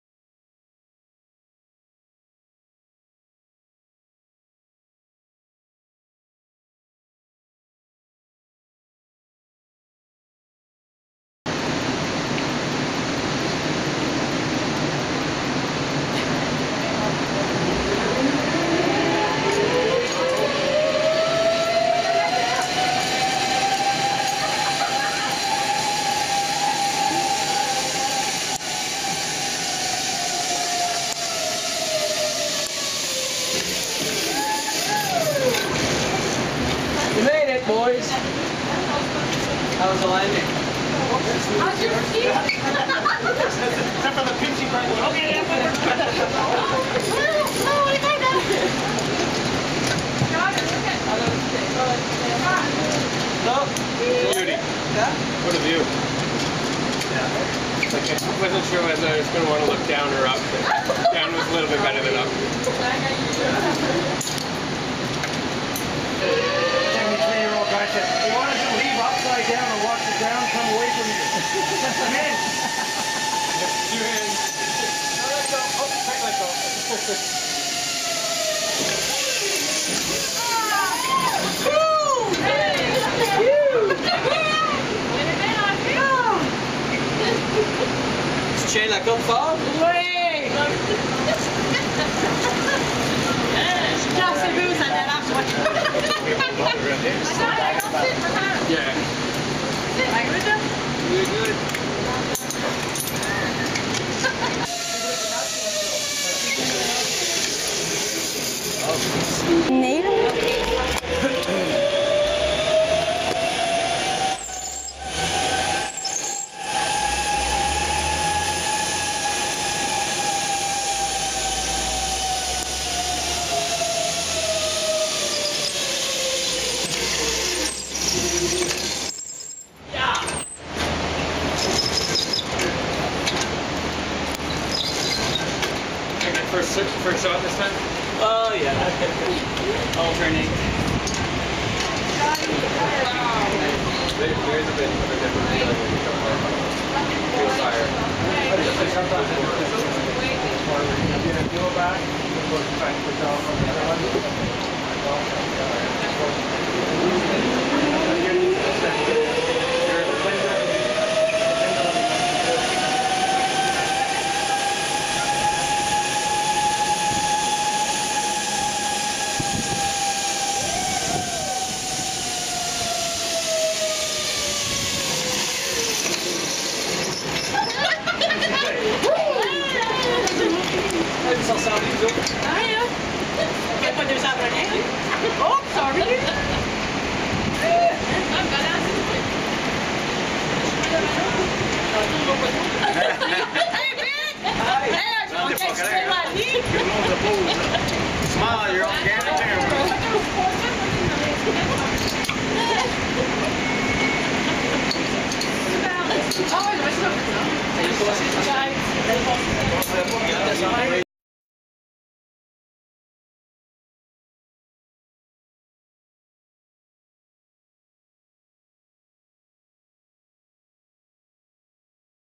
Re-Max Balloon Pilots,Scott MacRitchie & Tim Dale from Ottawa. 19-07-2011
et de Re-Max à Grand-Sault, France Pelletier et Murielle Moreau.